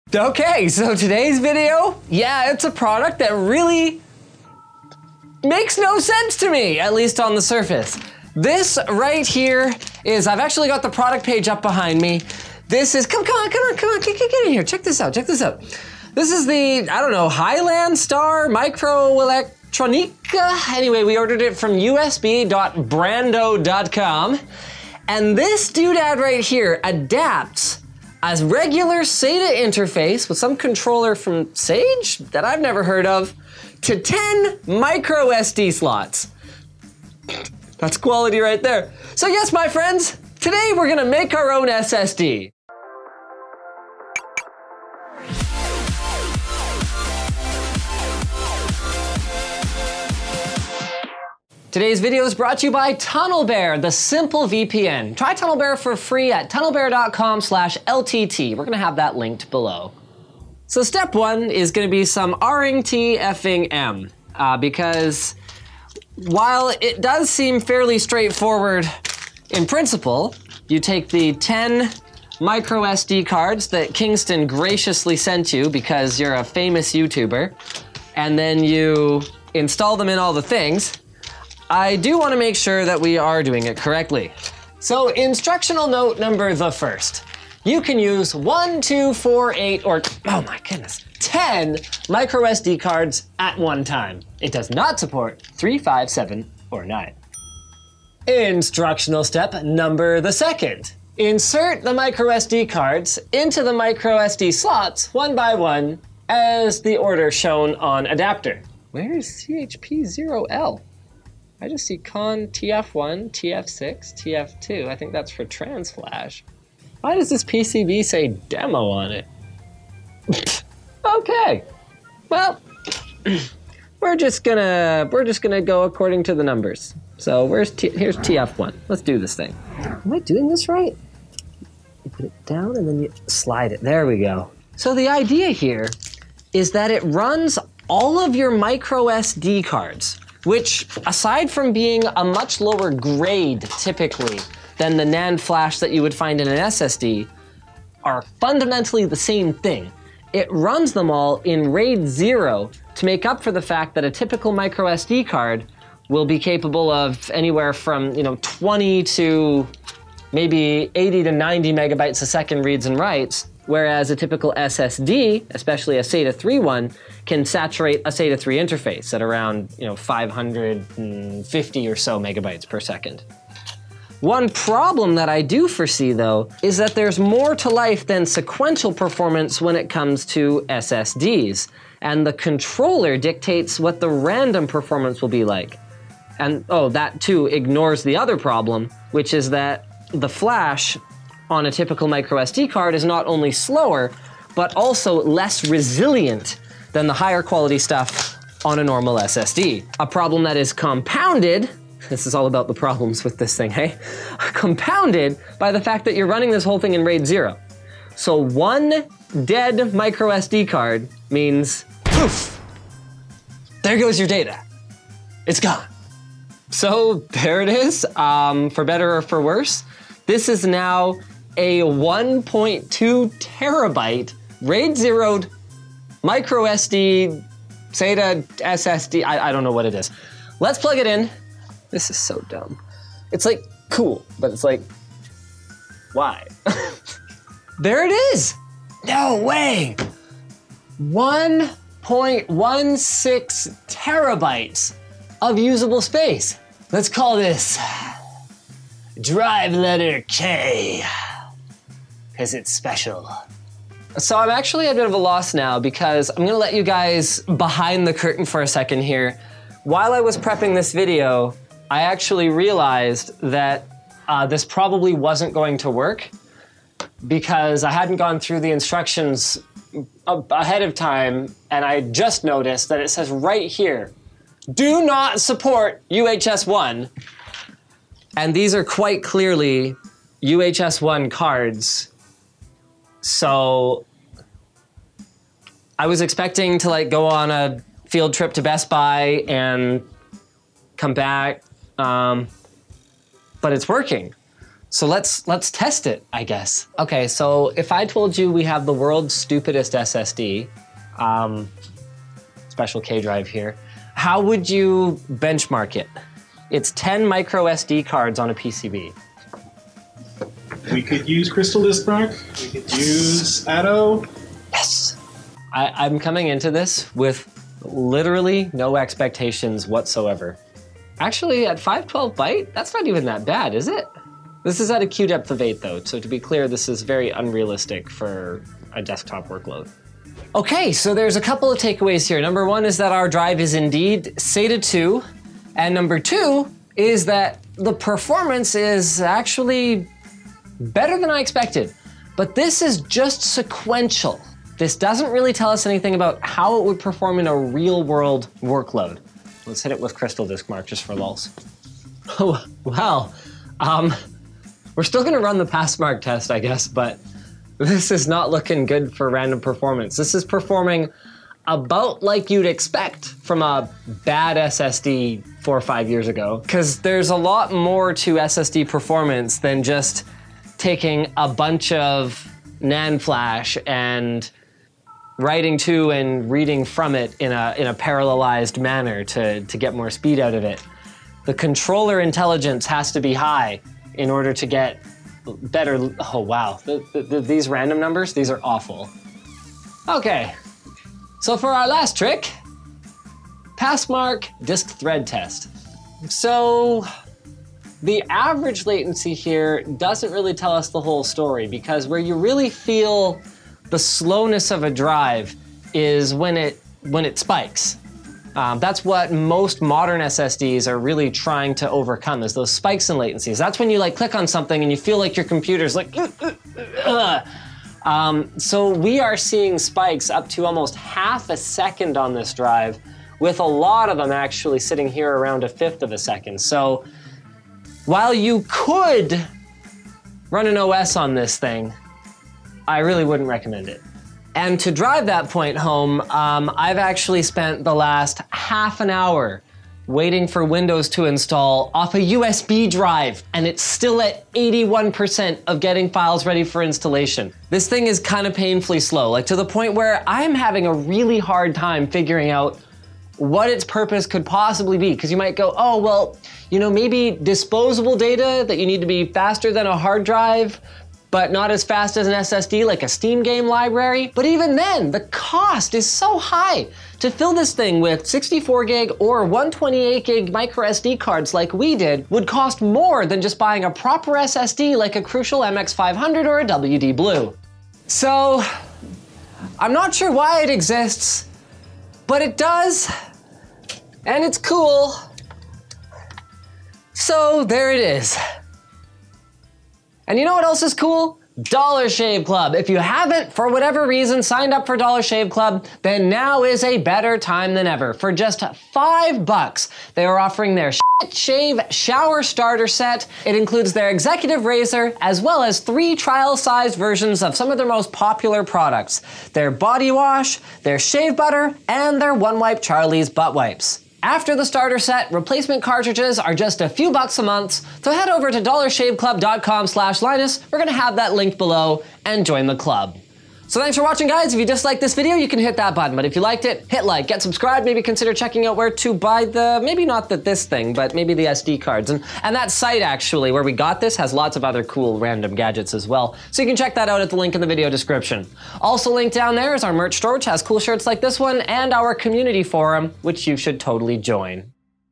DIY SSD made of SD Cards!
Does it make ANY sense to make your OWN SSD from Micro SD cards?